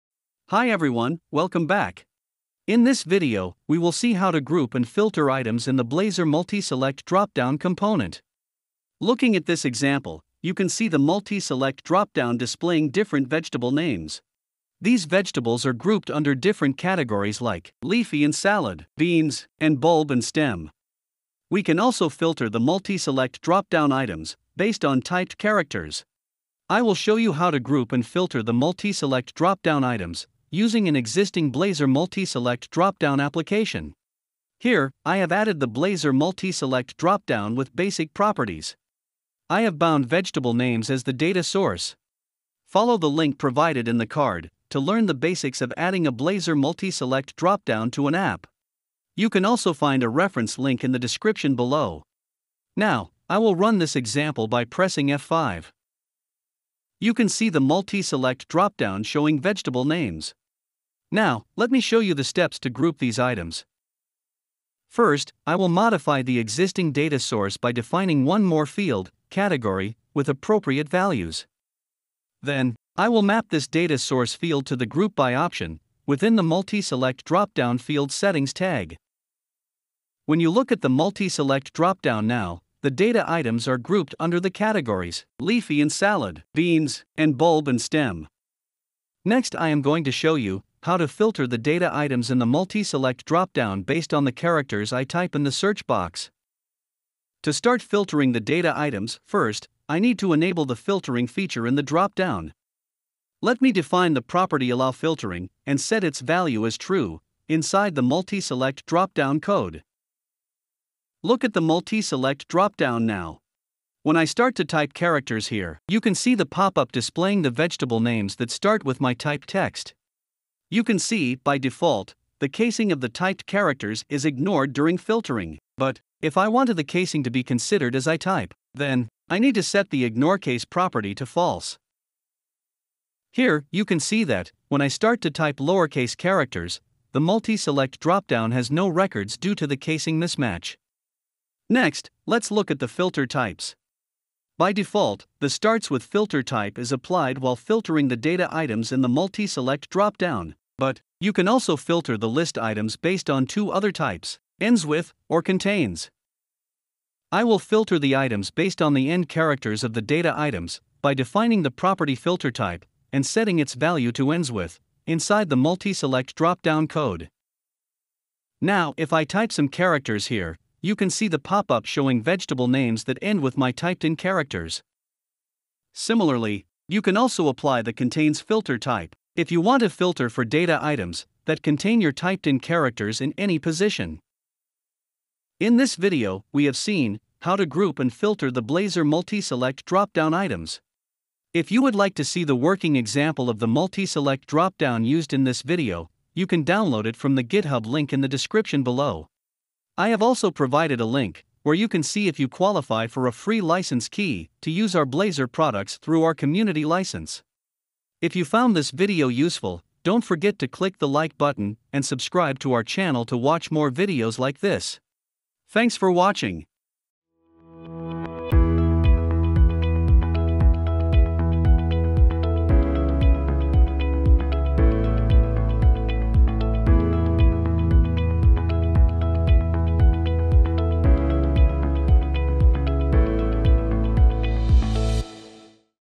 How to Filter and Group the Blazor MultiSelect Dropdown Items
In this video, you will learn how to group and filter Blazor MultiSelect Dropdown items. You will also see different filter types in the MultiSelect Dropdown and how to apply them.

If you need a trial license key, start your Blazor trial from your Syncfusion account. Then, you can obtain your trial license key from the downloads page.

BOOKMARK DETAILS
[00:00] Introduction
[01:05] Group items
[01:40] Apply a filter
[02:37] Change the filter type